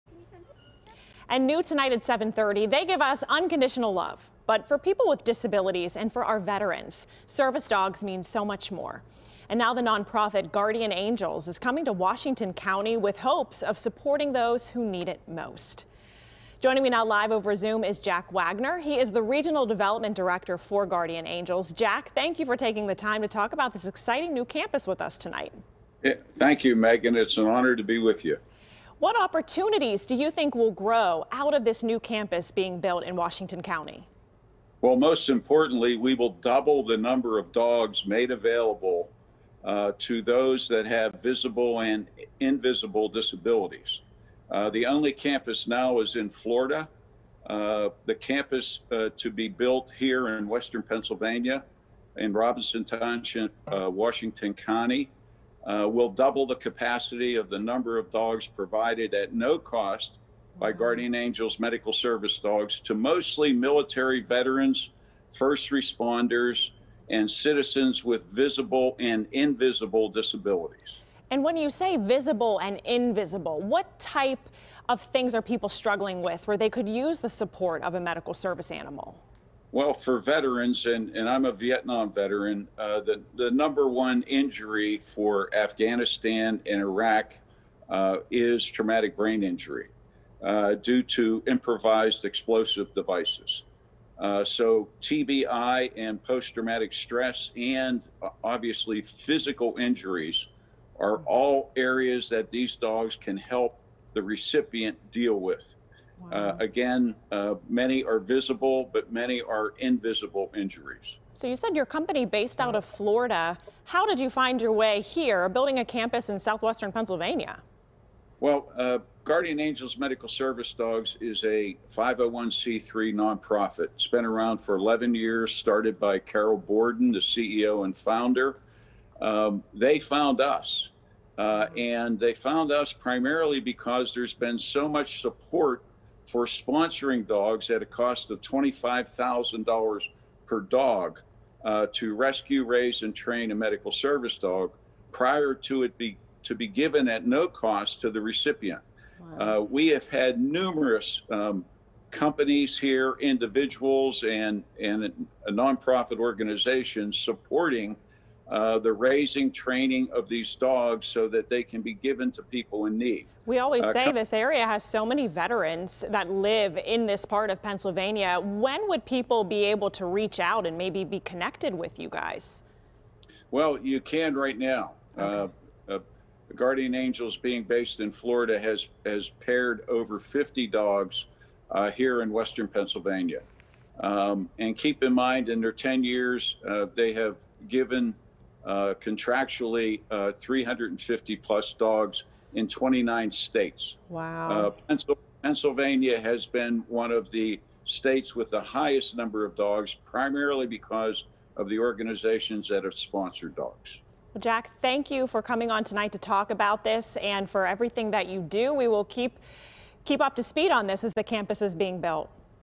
Guardian Angels Coming To Washington County
The service dogs nonprofit hopes to support those who need it the most.